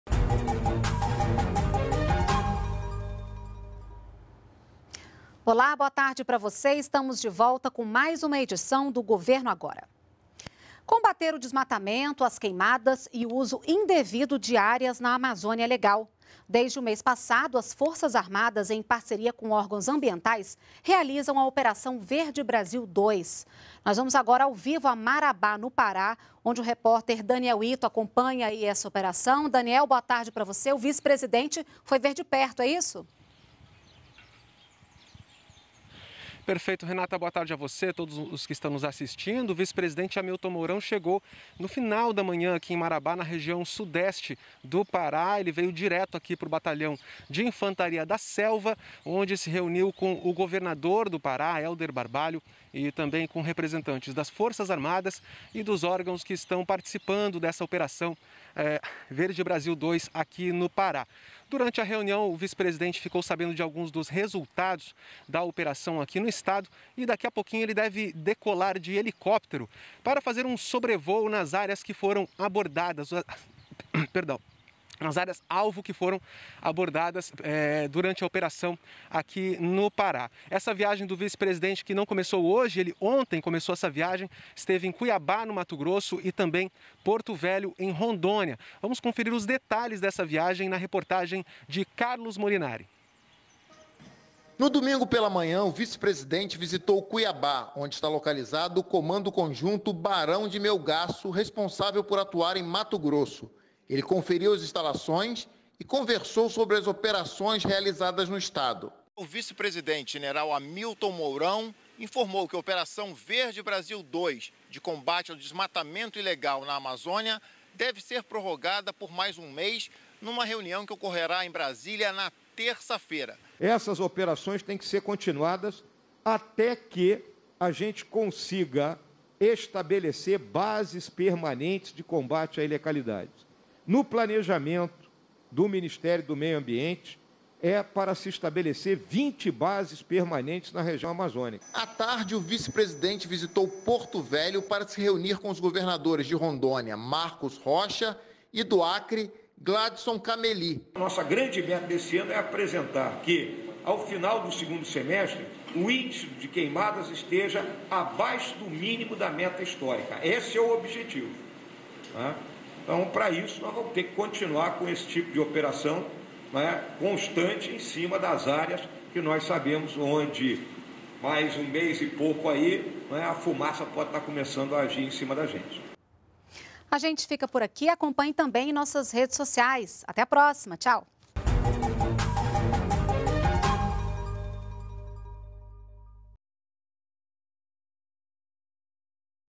Vice-presidente Hamilton Mourão visita ações da Operação Verde Brasil 2 no Pará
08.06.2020 - O vice-presidente Hamilton Mourão está em Marabá (PA) para ver de perto as ações da Operação Verde Brasil 2. Desde o mês passado, uma parceria das Forças Armadas com órgãos ambientais promove medidas para combater o desmatamento, as queimadas e o uso indevido de áreas na Amazônia Legal.